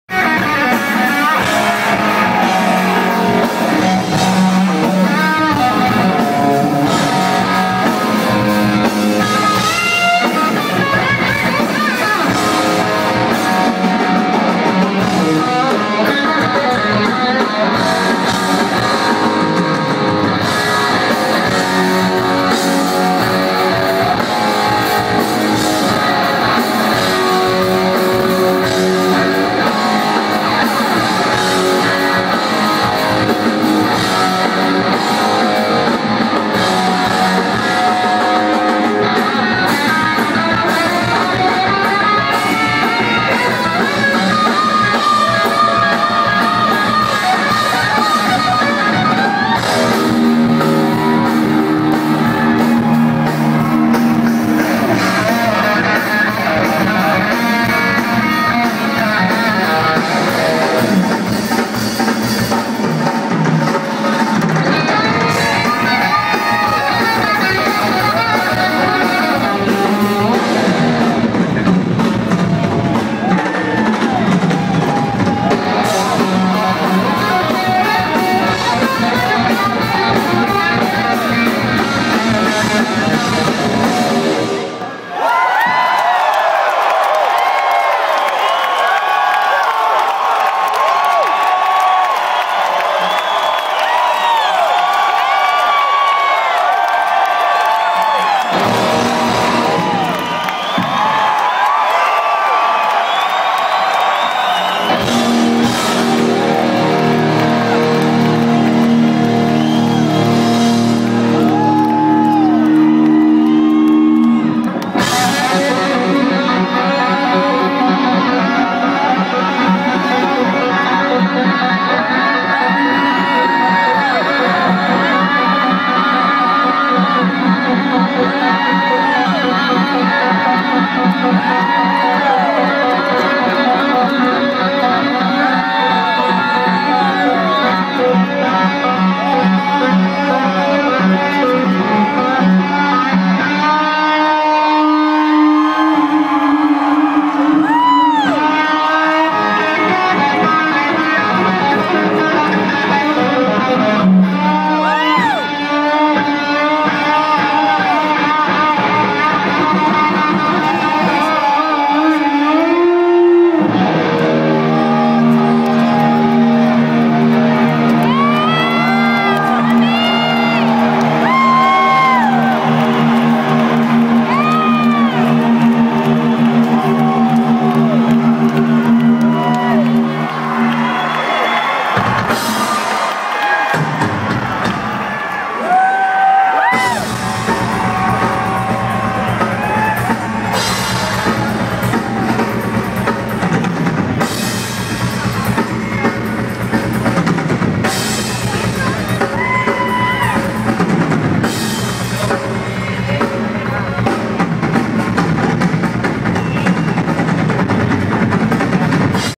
Kiss Eric & Tommy Solo Phoenix Arizona 09/24/2010
Flew From Alaska to Arizona to meet & see  Kiss.  What a great Show.  Eric & Tommy Jam together.